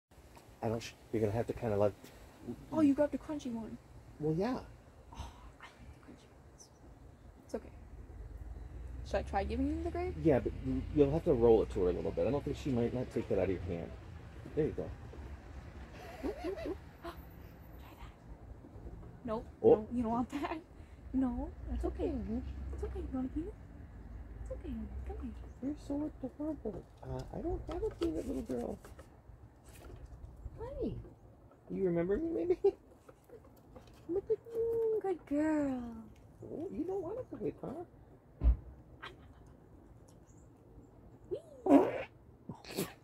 Sunshine the squirrel. :)
Sunshine is a very smart squirrel.  She knows that peanuts, almonds and avocado are very yummy.